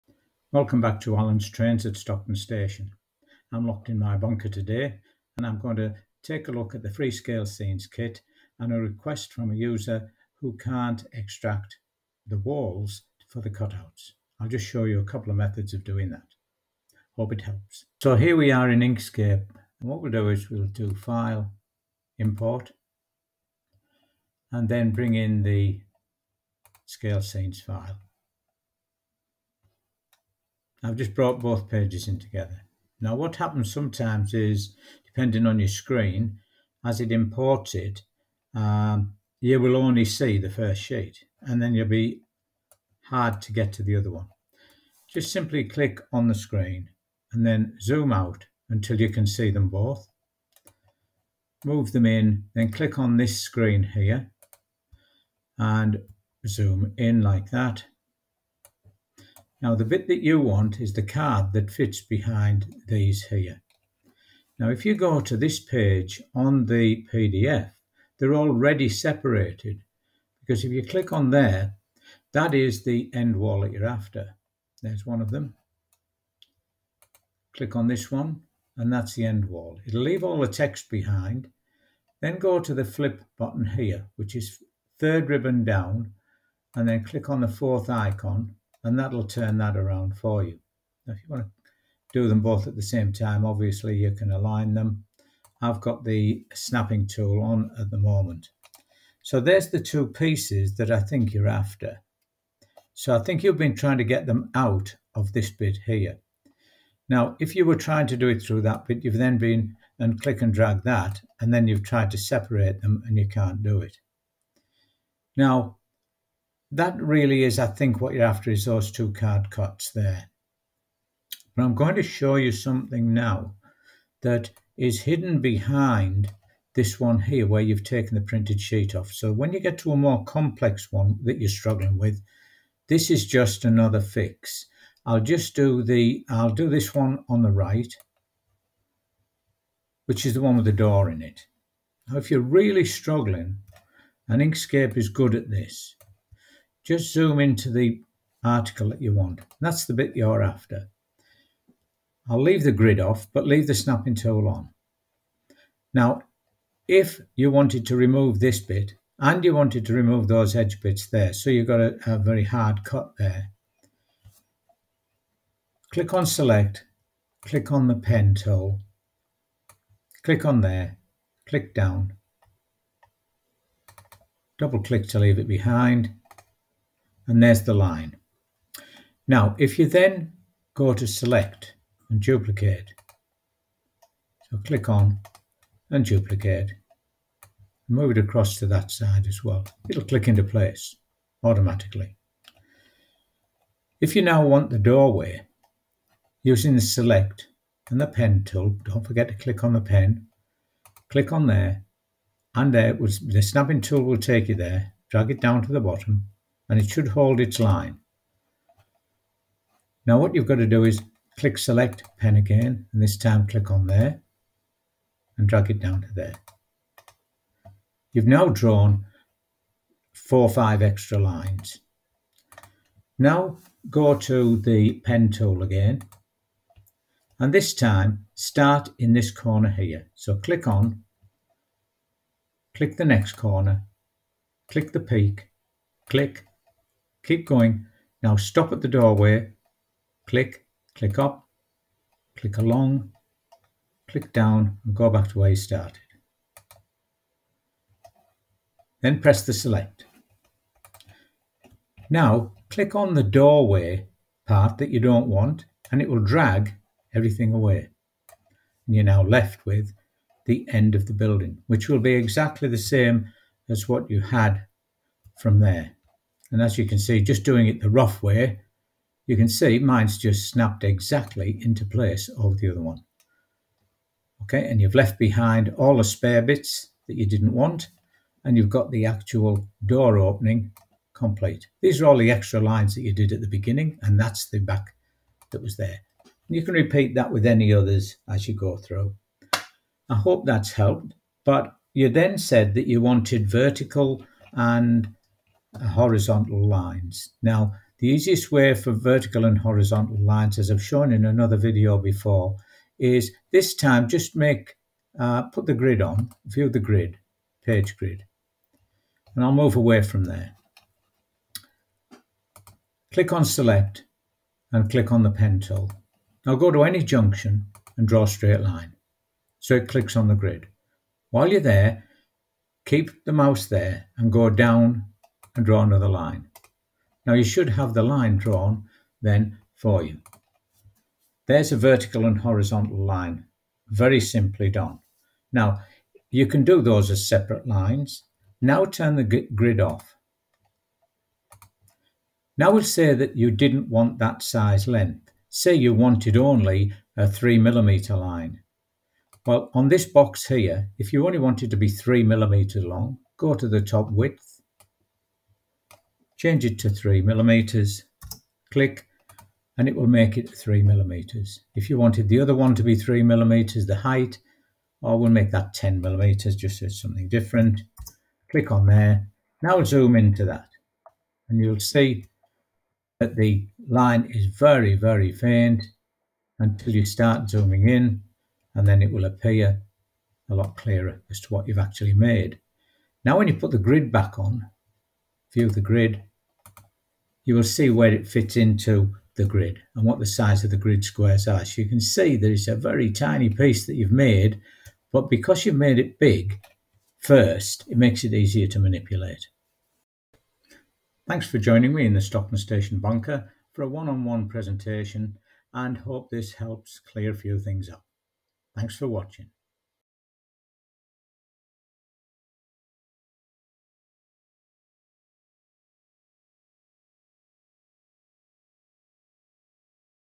Inkscape and Scalescenes Weighbridge to Cutting File (.svg)
This is a 'one on one' video on request from a new user of Inkscape, Scalescenes and a Cricut Maker 3 Cutting Machine.  The problem was 'how to get to an .svg file for the cutting machine".

I show how to import a PDF to Inkscape then extract the background for a cutting machine by two methods.  One of the methods can be used for more complex PDF imports.